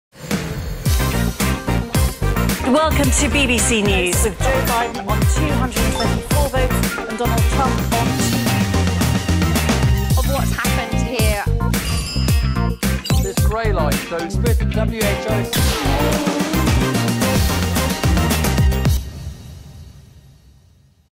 Arti: Cloud-based AR for broadcasters
Forget everything you know about augmented reality. See how easy, flexible, and affordable AR can be with Arti, the cloud-based AR platform.